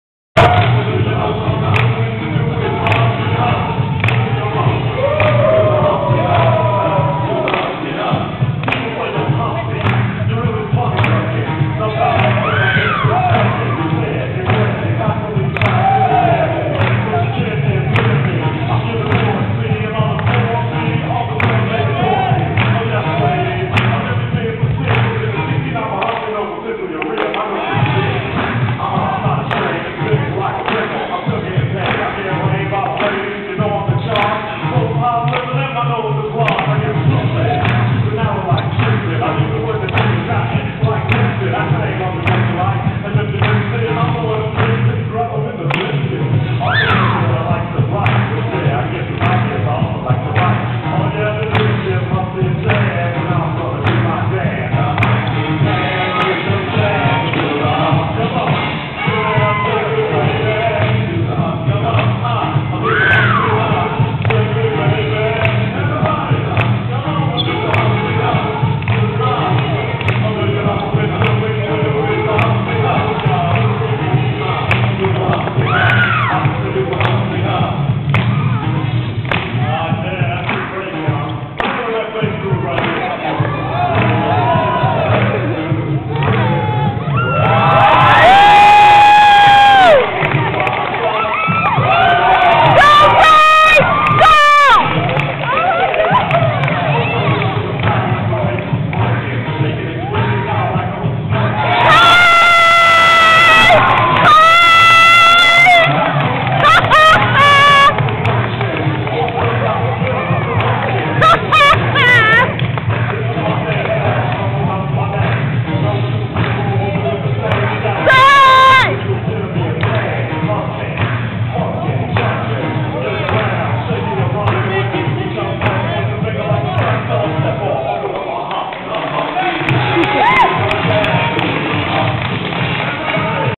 AACC HipHop Workshop
Hometown Heroes Hip Hop Dance Workshop at Anne Arundel Community College. Jamile McGee; 3rd place winner of 2005 "So You Think You Can Dance" reality show joins alumus Lance Guillermo from the movie "Step Up" and California dancer Chris Scott.  This is the end of the morning Jam Session of the workshop. Check out the nine year old handsome dude at the end of this video!